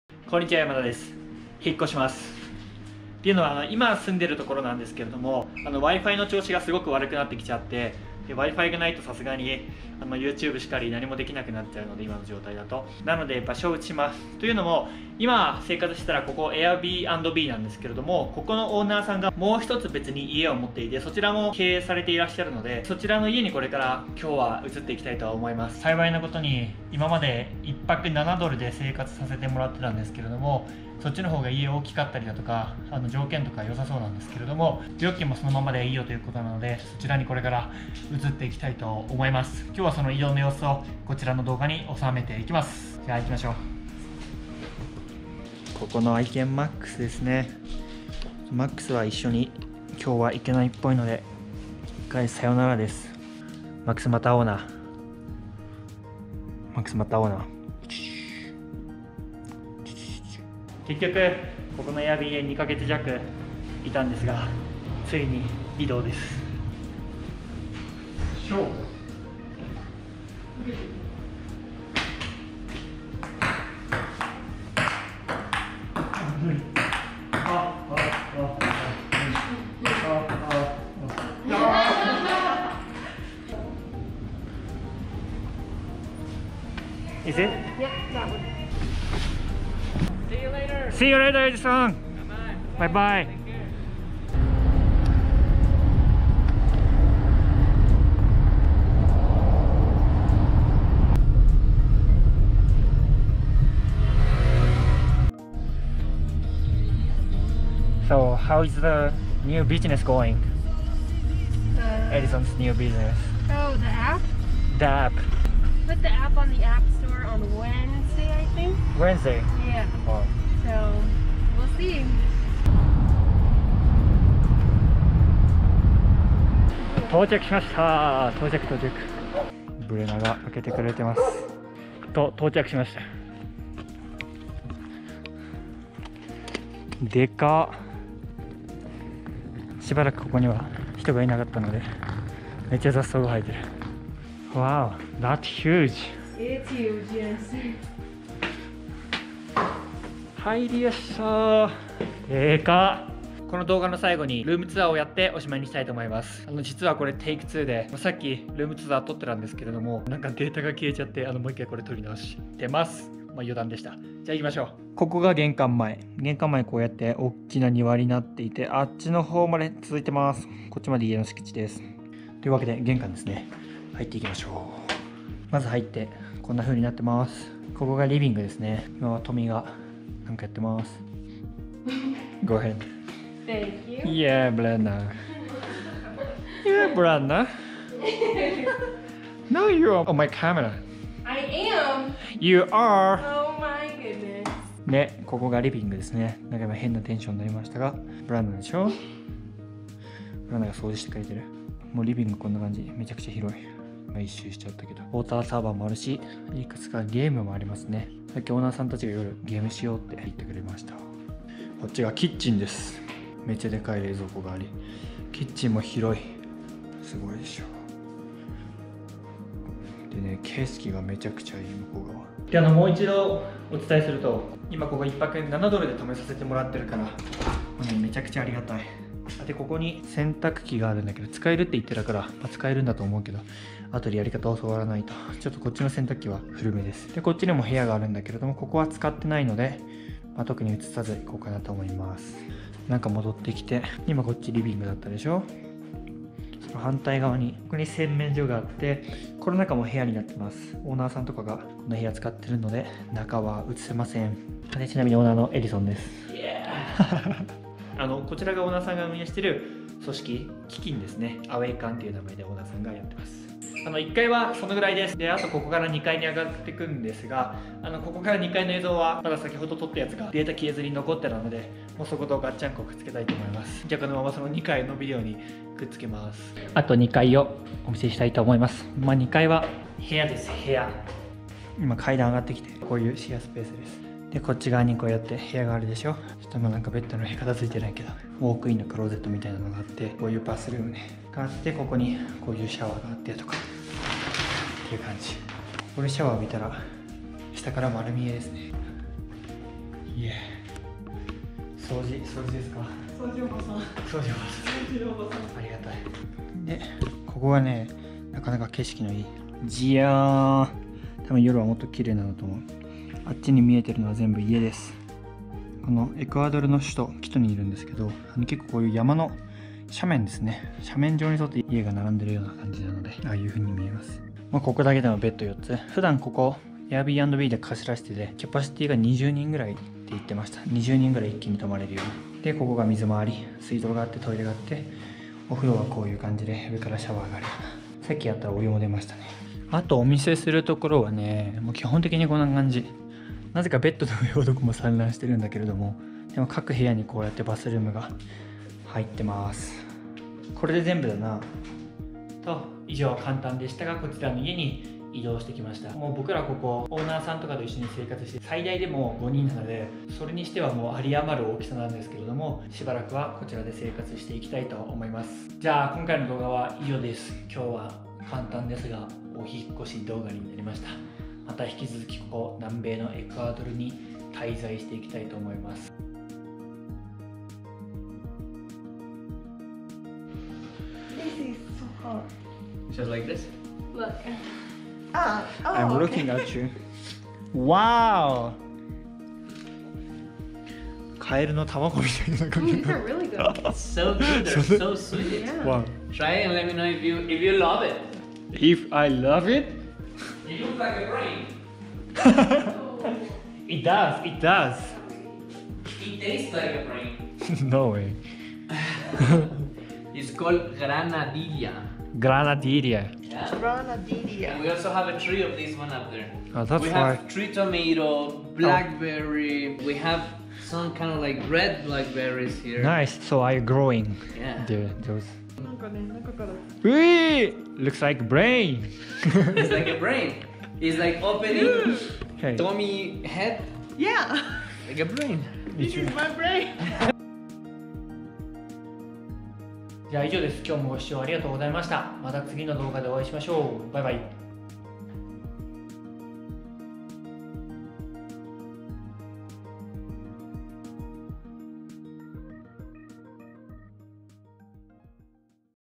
【Minimalist】Moving into a new house with one backpack
This channel is about a Japanese minimalist traveling around the world!!

★Today's Video★
I am moving into the different house with only one backpack which is of course easy to pack.
(I cannot keep traveling around the world because of coronavirus for a while. So I have been staying at an Airbnb in Ecuador. )

★About me(YAMADA)★
I am making videos about how a Japanese solo traveler spends a day.